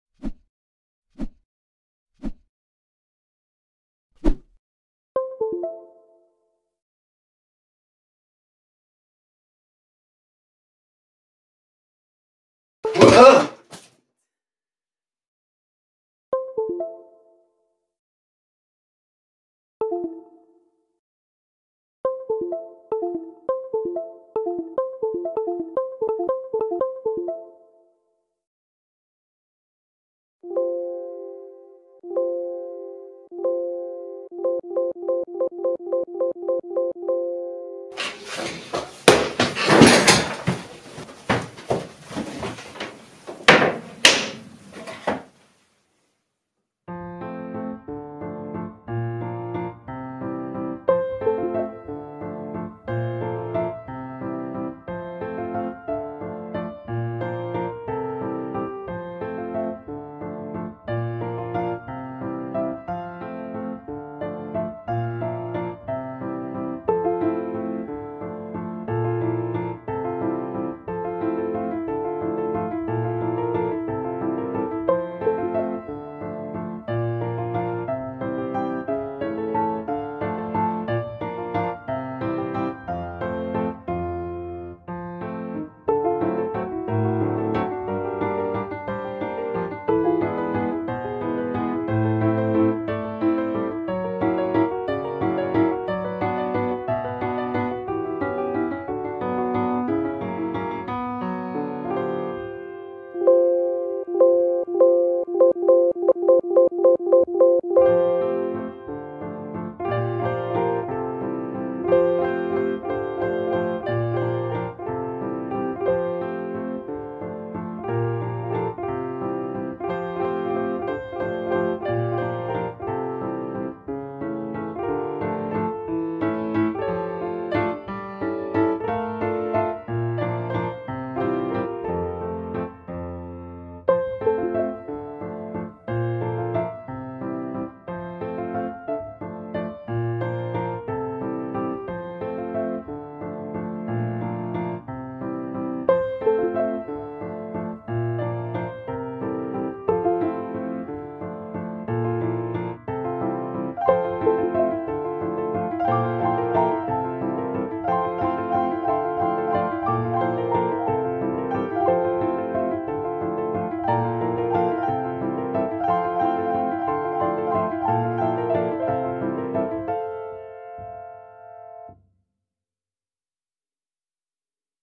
Windows 10 Waltz
Manchmal, wenn man im Homeschooling arbeiten will, hat Windows andere Pläne ¯\_(ツ)_/¯